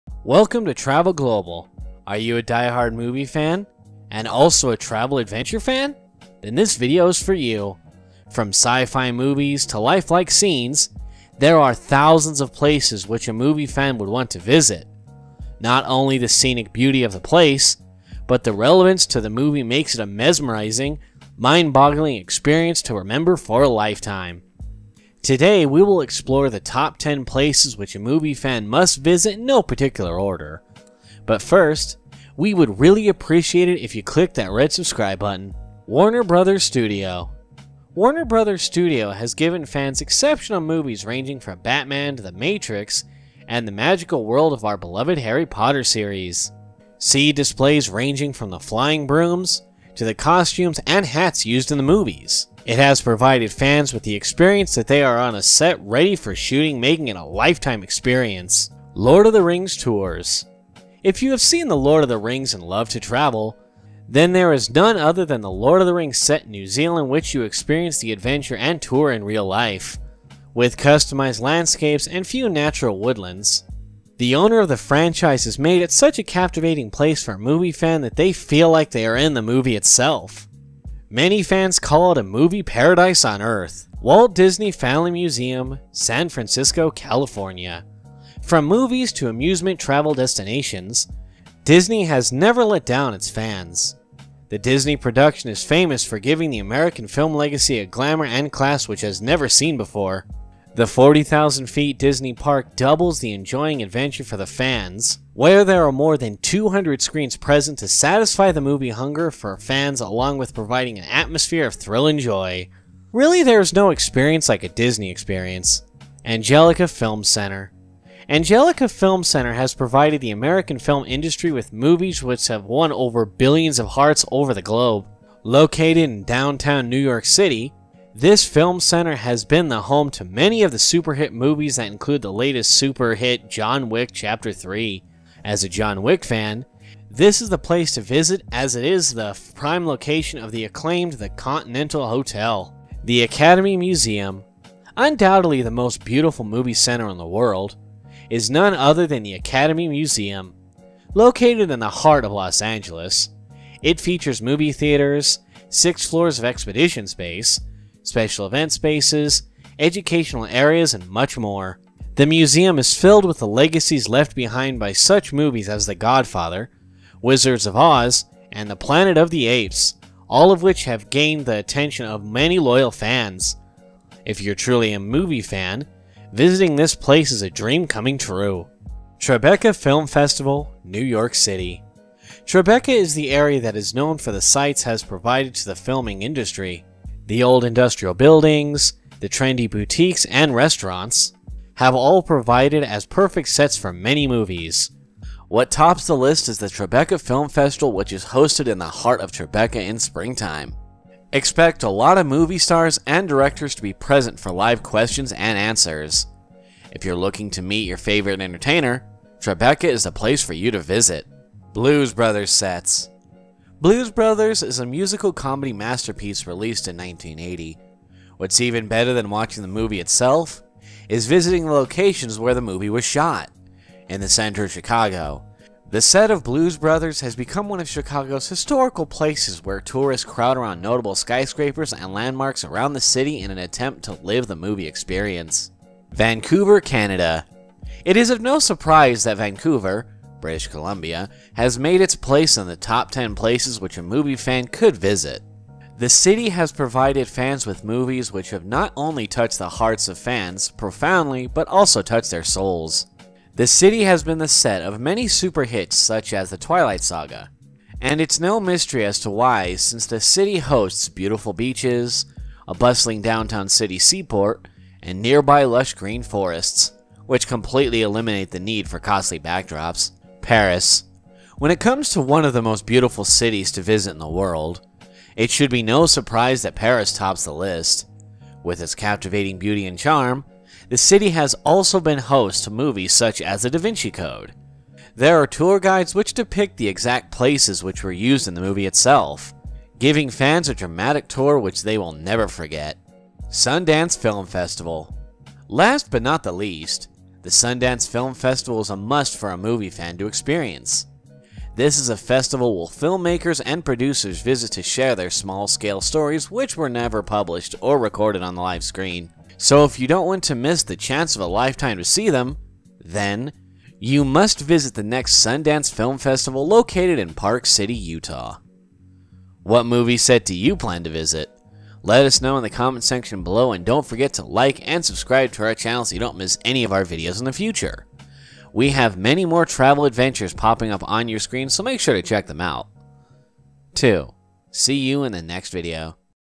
Top 10 MOVIE SETS Fans Must Visit! (Harry Potter, Lord of the Rings, Star Wars)
Are you a movie fan wishing to explore your favorite movie set? Check out these top 10 places movie fans must visit around the globe! Especially for Star Wars, Harry Potter, and Lord of the Rings fans!

We hope this video gives you entertainment and great ideas for future travel.

We post weekly on Tuesday at 12pm EST and Friday at 5pm EST.